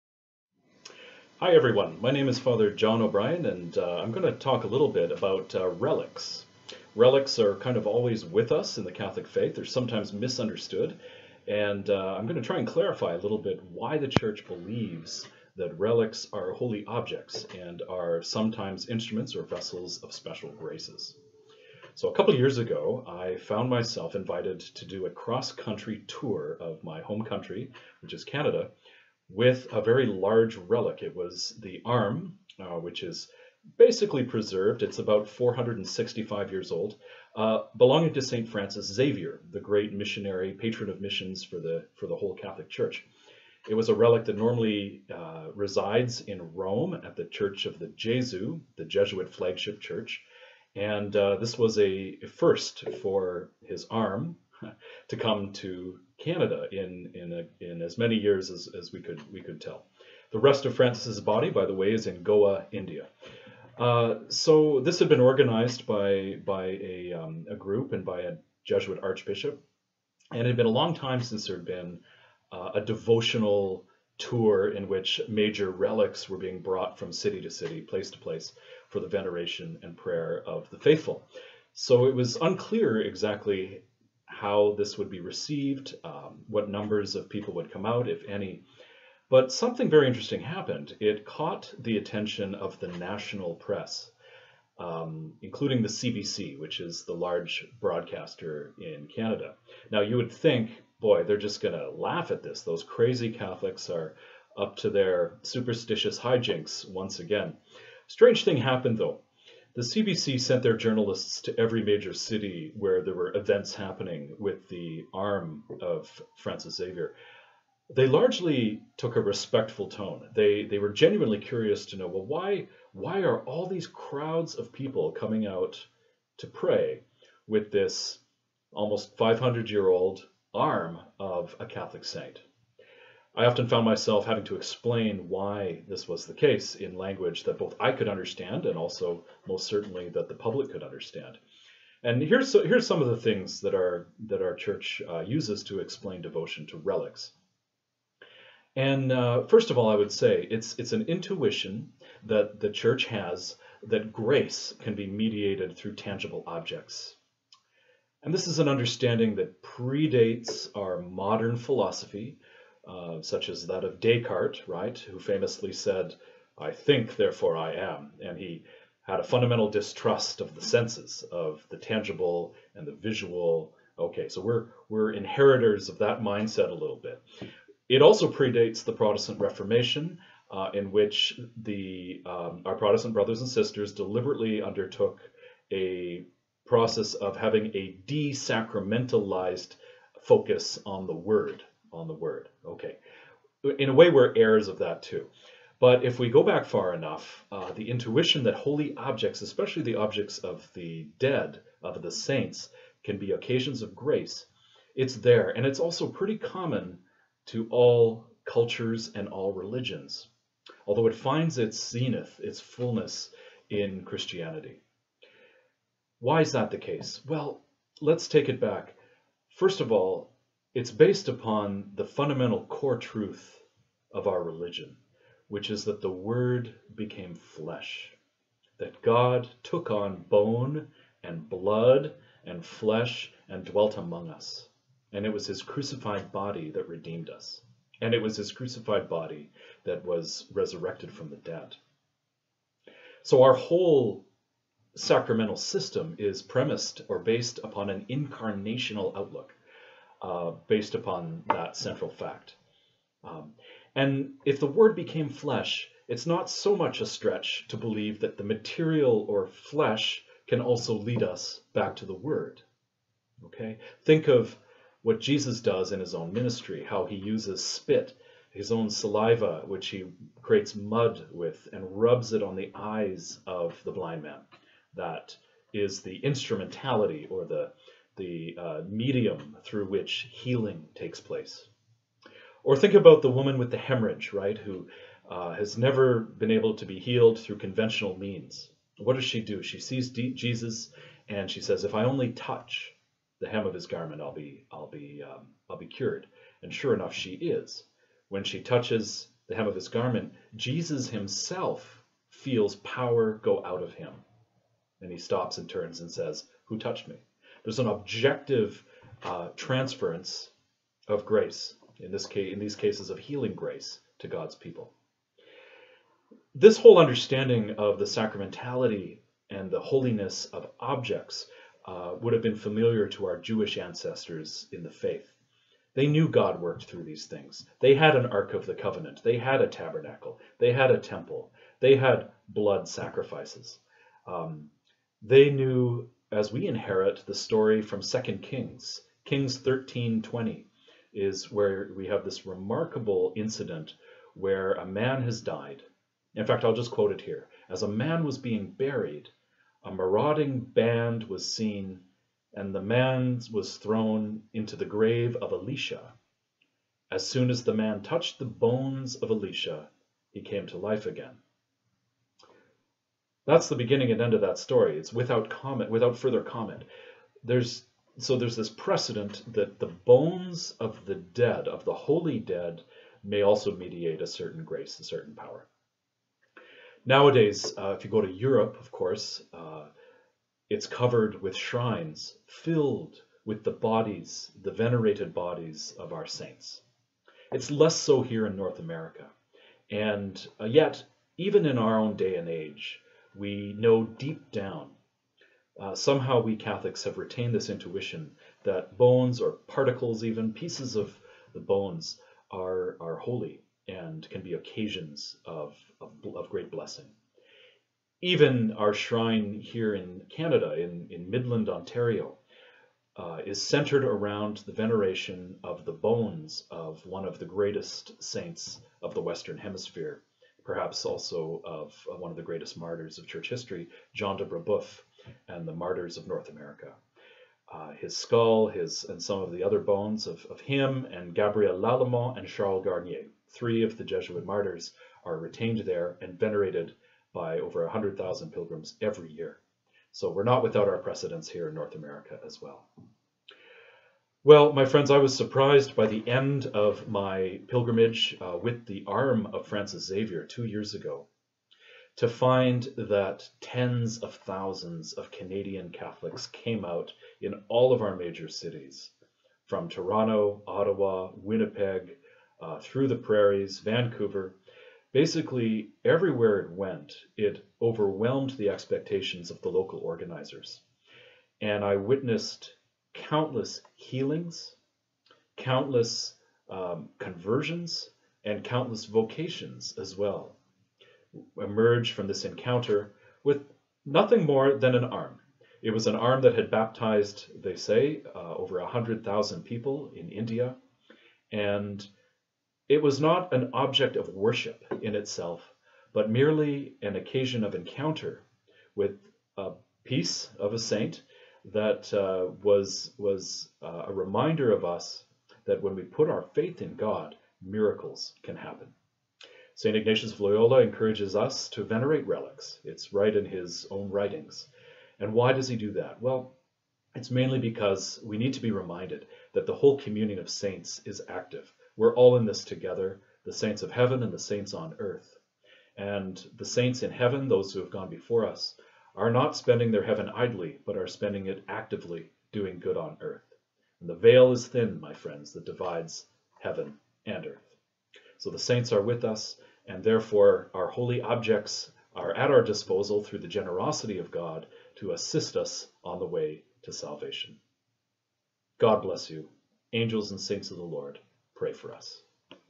Understanding Relics - Fr. John O'Brien
Fr. John O'Brien, S.J. toured the country with the 465-year-old arm of St. Francis Xavier, and describes in brief a Catholic understanding of relics. Special for Catholicity benefactors. To read some of the first-hand stories of graces that preceded from the St. Francis Xavier relic tour in Canada,